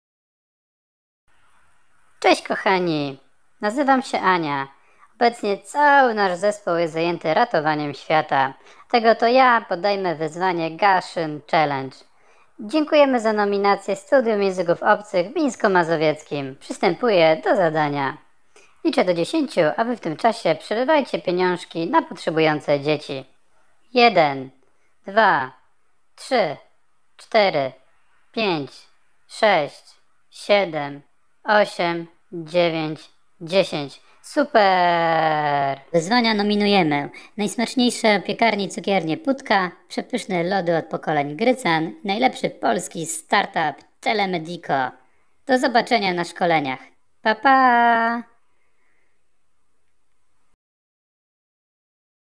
Gaszyn Challenge - Eskulapa
Obecnie nasz zespół jest niedostępny, dlatego zastąpiła nas niezawodna Ania! 😎

Dziękujemy za nominację Studium Języków Obcych w Mińsku Mazowieckim

My nominujemy:
🥖 Piekarnie Cukiernie Putka
🍦 Grycan
👨‍⚕️ Telemedi.co

Do dzieła! 👏 Zasady są proste: nominowani muszą wykonać 10 pompek lub przysiadów, nagrać film i umieścić go na swojej stronie. Jeśli nominowani nie wykonają zadania wpłacają 10 zł, jeżeli wykonają zadanie 5 zł na konto z poniższego linku. Czas na wykonanie 48 h.

Ponieważ cel dla Wojtusia został osiągnięty, jest nowy link do wpłat, dla malutkiej Sandry: